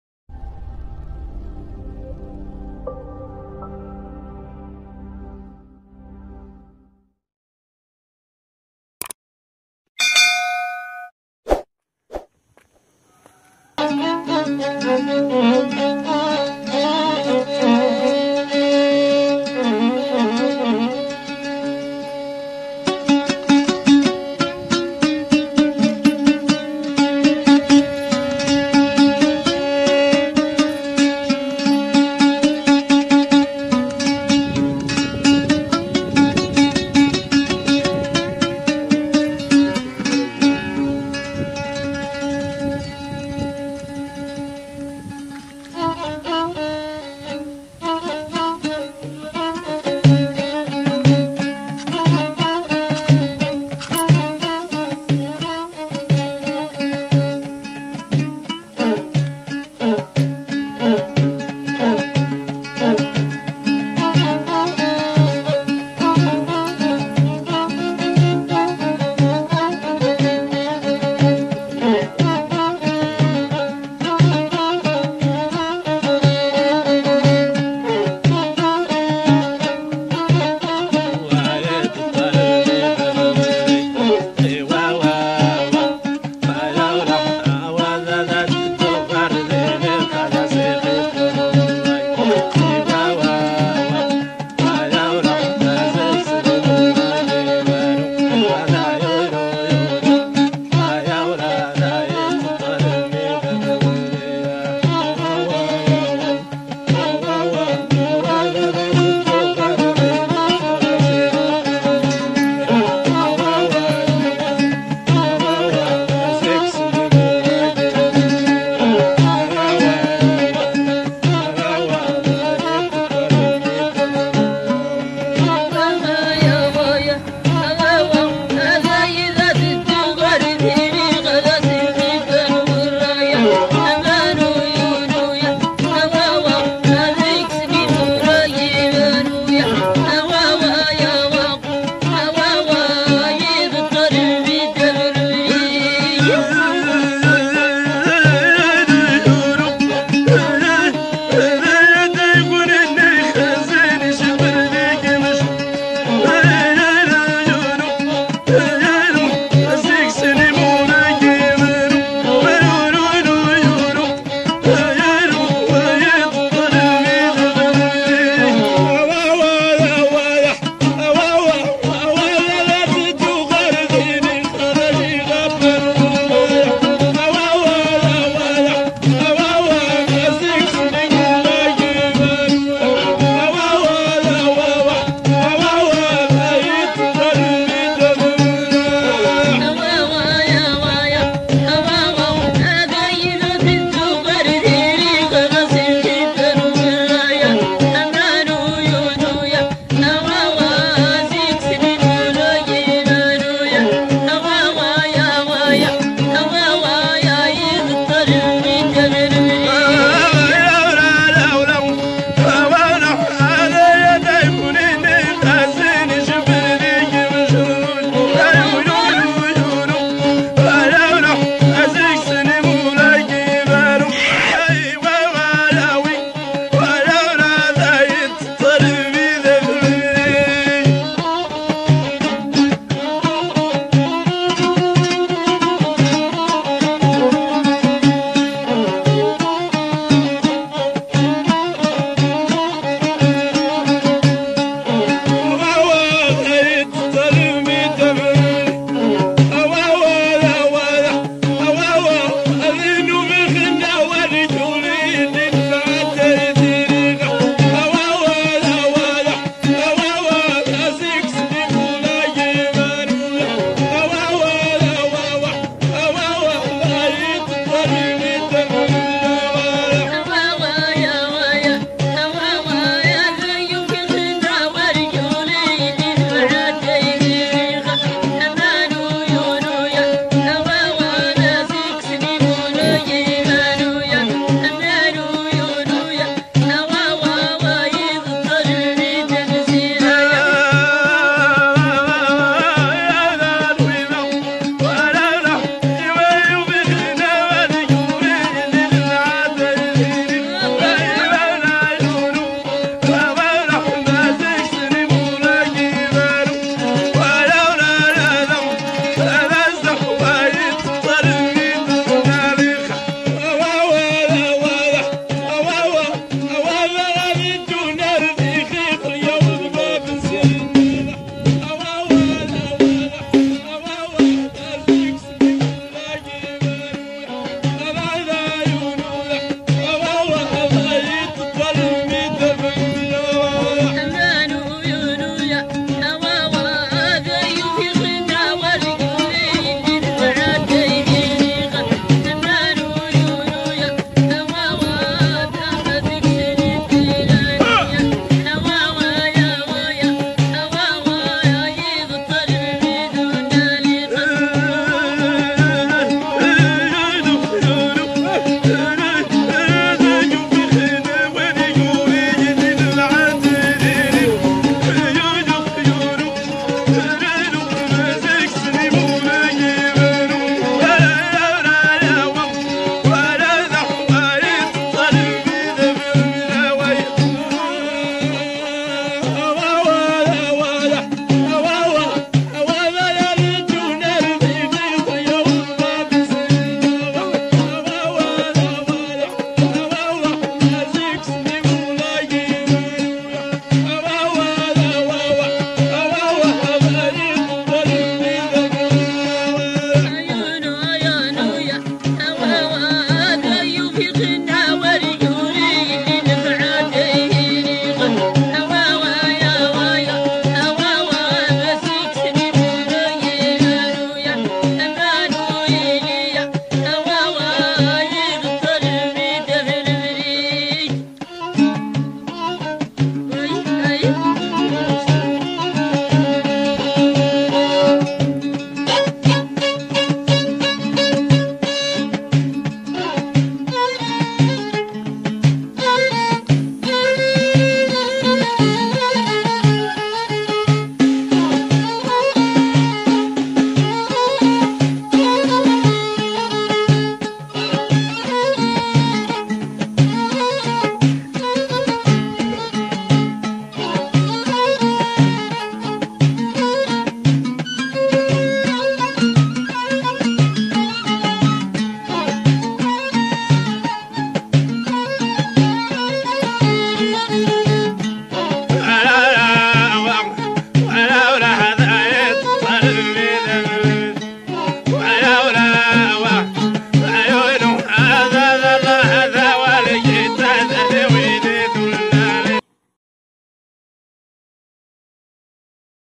طبعة خلبة مع المسقة
موسيقى التقليدية المعتمدة على الالات الوترية (كمبري) والالات النفخ الخشبية (الناي) مما يدل على تأثرها بالموسيقى الإفريقية بسبب الهجرة البشرية الآتية من افريفيا السوداء في العصر السعدي.